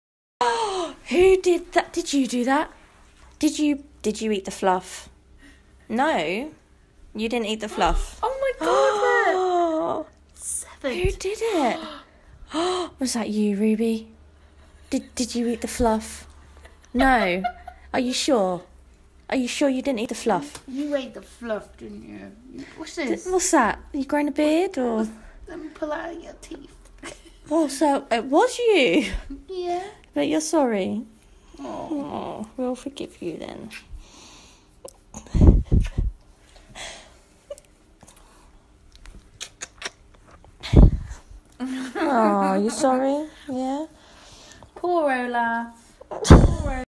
WORLDS MOST DANGEROUS DOG... SAVAGE STAFFY, lucky to escape this one!.
Meet The staffy ... the dangerous, scary STAFFY named Ruby...  warning- content may be too cute to handle...